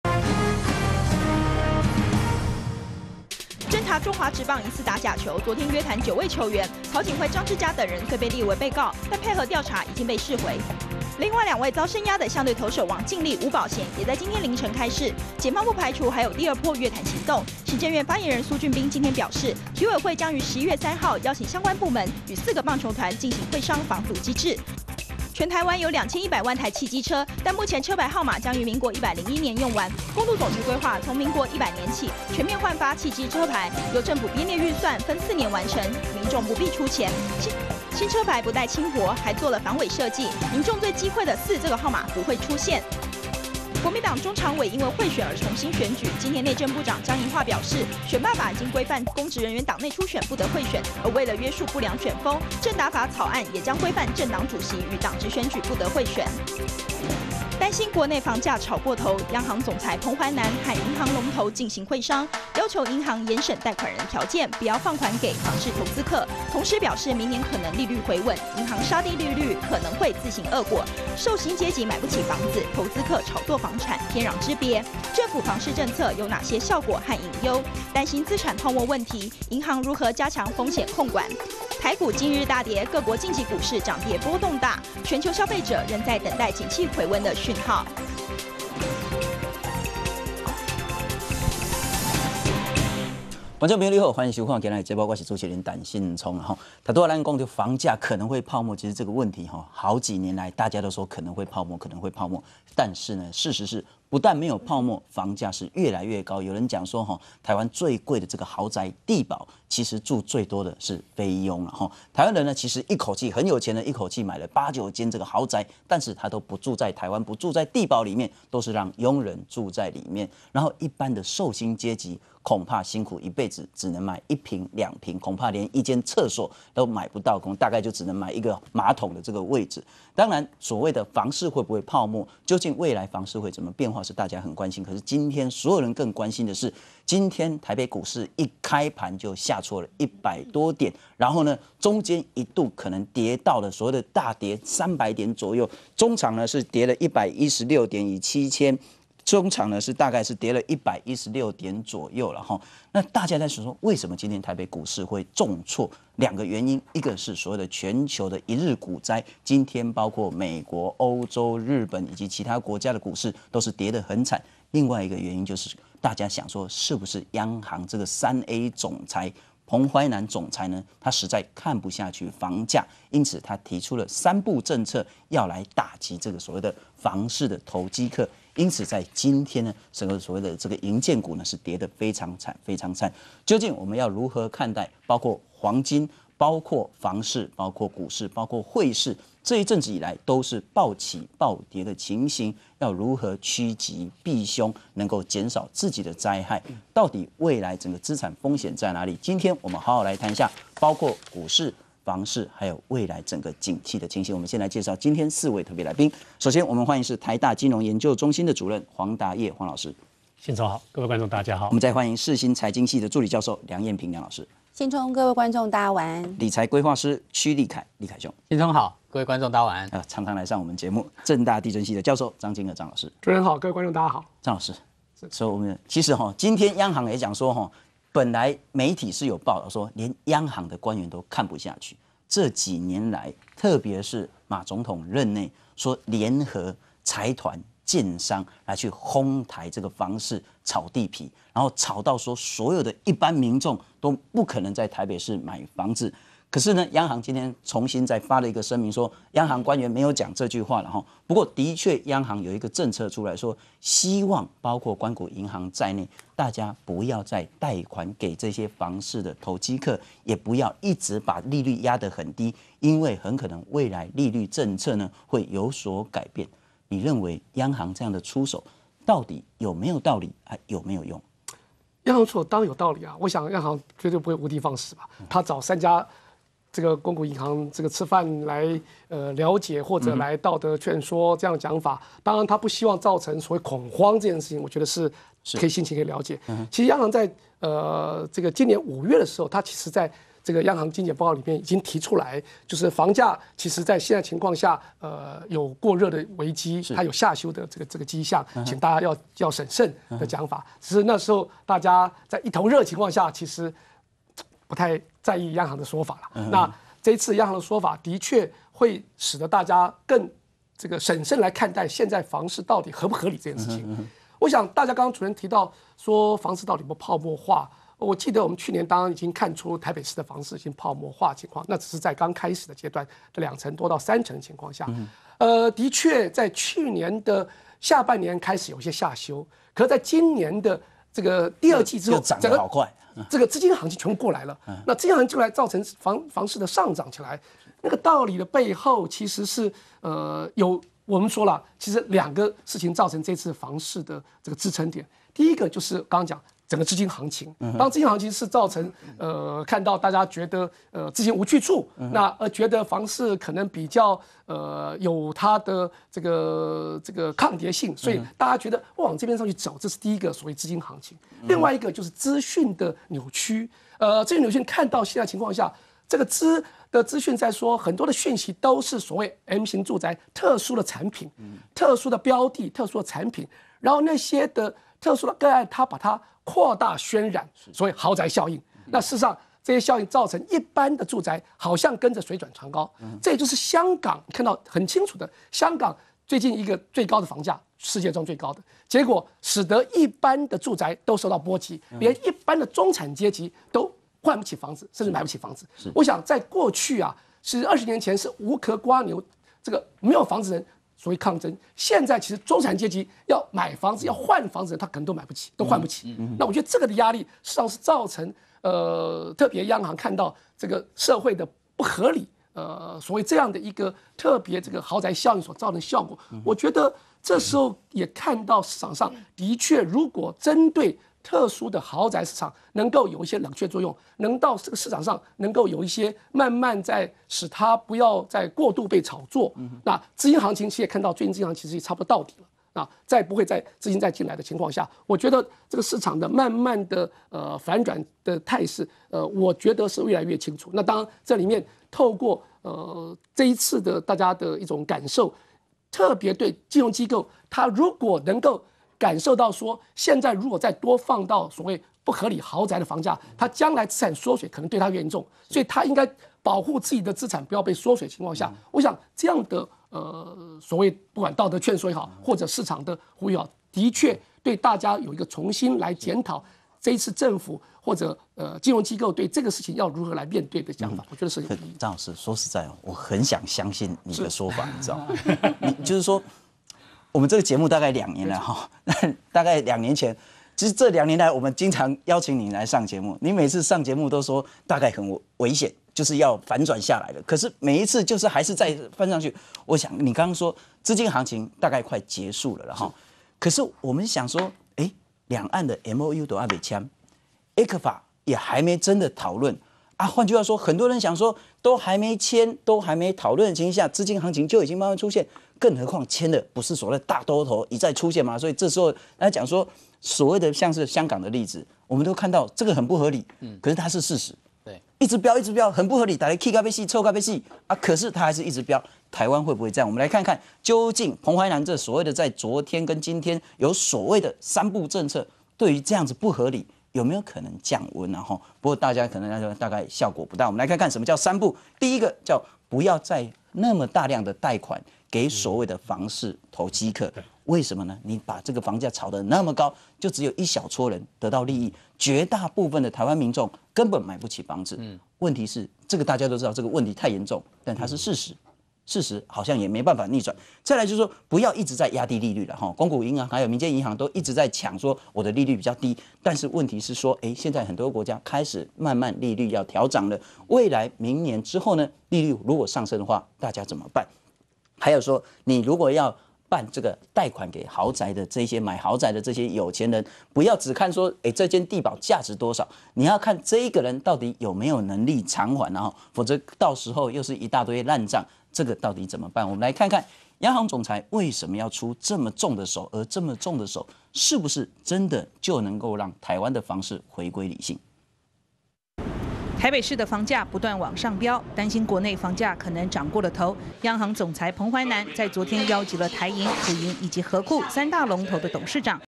1029政府建商合炒地皮？央行真能抑制房價？
政府房市政策有哪些效果和隱憂？彭淮南呼籲銀行加強風險控管，銀行做得到嗎？政府真的想修正房價嗎？政府角色該如何？

全球股市重挫，台股開盤大跌，一度跌破季線，大跌近300點，終場下跌178.26點。全球股災！股市暴起暴跌，原因為何？全球消費信心指數反彈，全球景氣真的有回溫訊號？